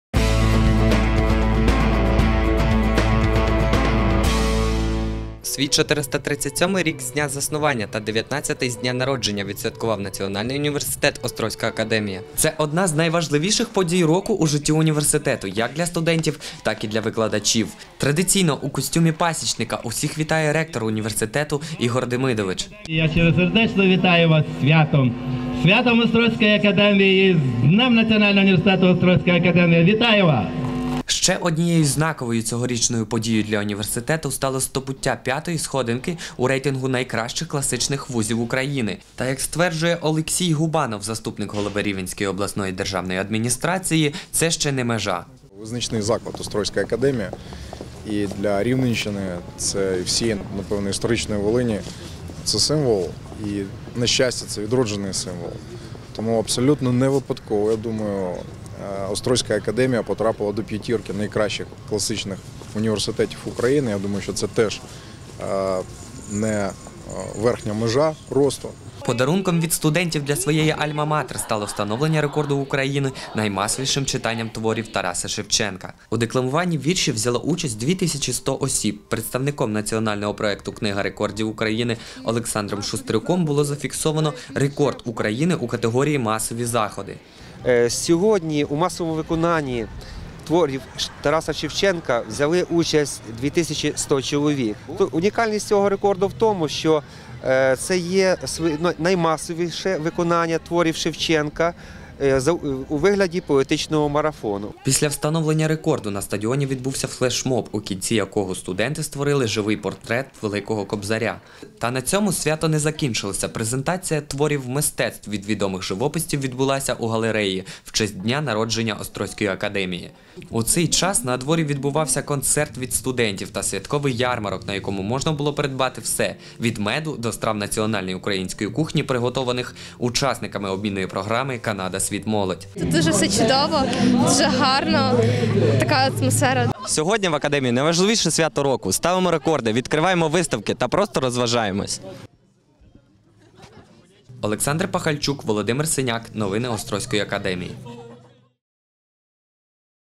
Острозька академія відсвяткувала день народження
Цьогоріч першого жовтня Національний університет «Острозька академія» відсвяткував 437 років від дня заснування та 19 з моменту відродження